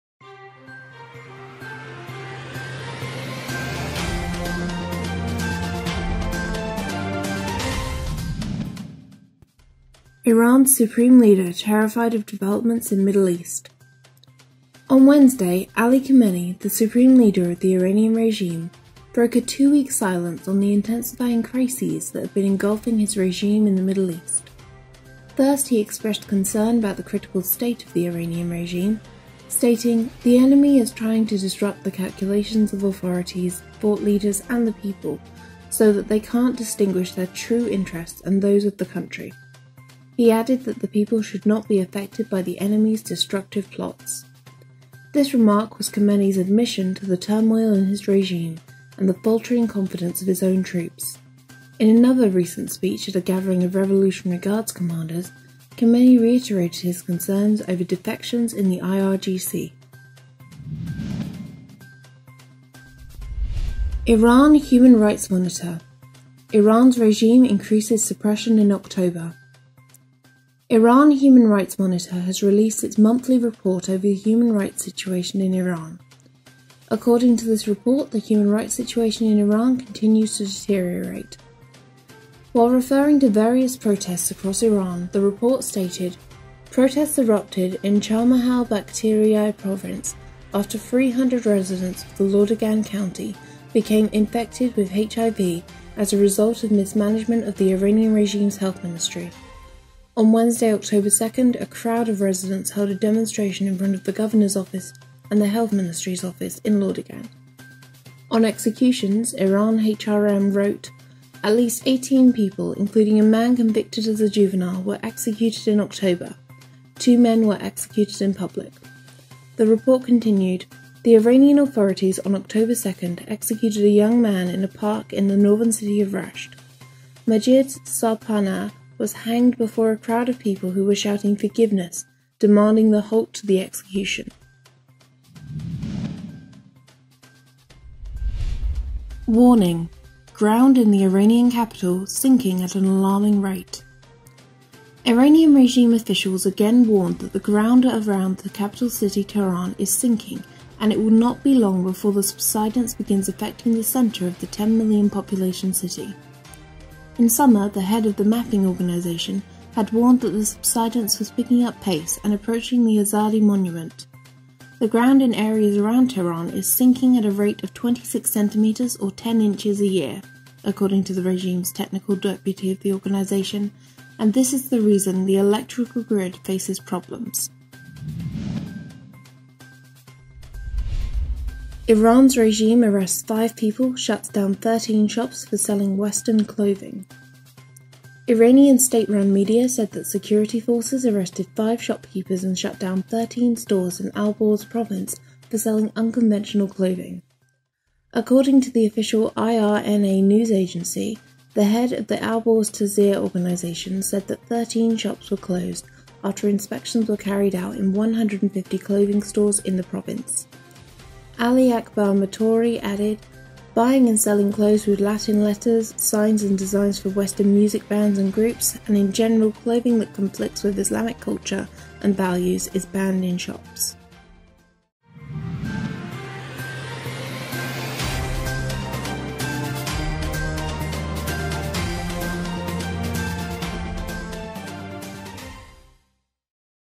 Iran news in brief, November 1, 2019
Iranian Regime’s Supreme Leader Terrified of Developments in Middle East

On Wednesday, Ali Khamenei, the supreme leader of the Iranian regime, broke a two-week silence on the intensifying crises that have been engulfing his regime in the Middle East region.
First, he expressed his concern about the critical state of the Iranian regime, stating: “The enemy is trying to disrupt the calculations of authorities, thought leaders and the people so that they can’t distinguish their true interests and those of the country,” Khamenei said, adding that the people should not be affected by the “enemies’ destructive plots.”
This remark was in fact Khamenei’s admission to the turmoil in his regime and the faltering confidence of his own troops. In another recent speech at a gathering of Revolutionary Guards commanders, Khamenei had reiterated his concerns over defections in the ranks of the IRGC.

Iran Human Rights Monitor: Iran's Regime Increases Suppression in October

Iran Human Rights Monitor has released its monthly report over human rights situation in Iran. According to this report, the human rights situation in Iran continues to deteriorate.
While referring to various protests across Iran the report stated: "Protests erupted in Charmahal Bakhtiari province after 300 residents of the Lordegan county became inflicted with HIV as a result of mismanagement of the Iranian regime’s health ministry. On Wednesday, October 2, a crowd of residents, held a demonstration in front of the governor’s office and the health ministry’s office in Lordegan."
On executions, Iran HRM wrote: “At least 18 people including a man convicted as juvenile, were executed in October. Two men were executed in public." The report continued: "The Iranian authorities on October 2, executed a young man in a park in the northern city of Rasht. Majid Sarpanah was hanged before a crowd of people who were shouting “forgiveness”, demanding the halt to the execution."

Warning: Ground in the Iranian Capital Sinking at Alarming Rate

Iranian regime officials again warned that the ground around the capital city Tehran is sinking and it will not be long before the subsidence begins affecting the center of the 10-million population city.
In the summer, the head of the Mapping Organization had warned that the subsidence was picking up pace and approaching the Azadi monument.
The ground in areas around Tehran is sinking at a rate of 26 centimeters or 10 inches a year, according to the regime's technical deputy of the organization, and this is the reason the electrical grid faces problems.

Iran’s Regime Arrests 5 People, Shuts Down 13 Shops for Selling “Western” Clothing

Iranian state-run media said that security forces arrested five shopkeepers and shut down 13 stores in Alborz province for selling “unconventional clothing”.
According to the official IRNA News Agency, the head of the Alborz Tazir Organization said that the 13 shops were closed after inspections were carried out in 150 clothing stores in the province.
“Buying and selling clothes with Latin letters, signs and designs for Western music bands and groups and in general, clothing that conflicts with Islamic culture and values is banned in shops,” Ali Akbar Mokhtari added.